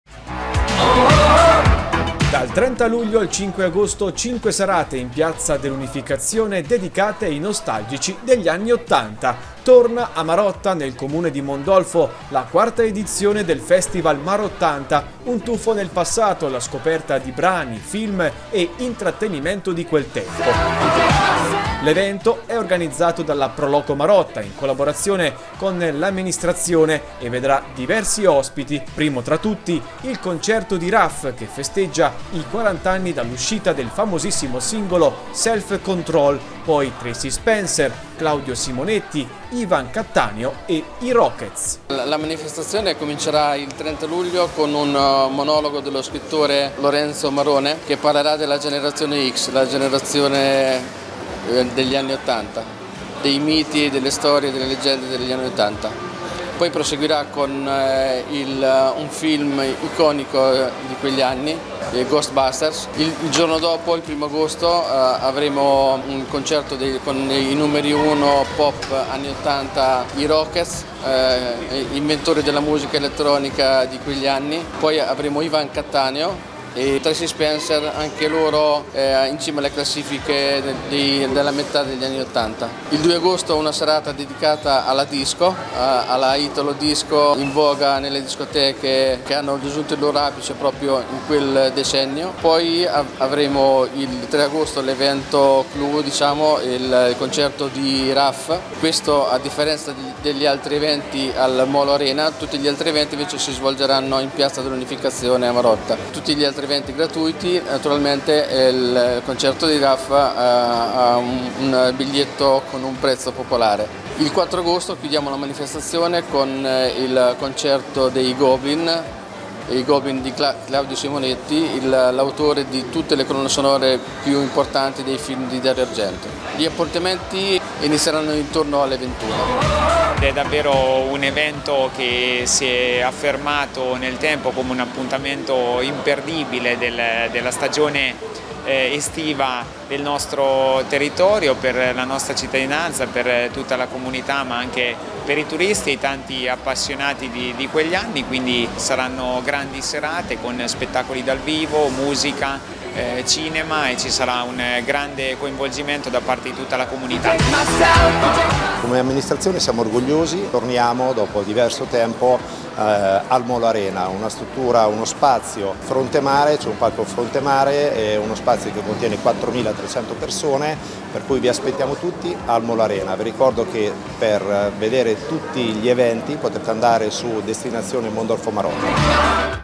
A Marotta un tuffo nel passato con 'Mar80', dal 30 luglio al 5 agosto
Tracy Spencer, Claudio Simonetti, Ivan Cattaneo, i Rockets e il concerto di Raf: a Marotta di Mondolfo torna la 4ª edizione di “Mar80”.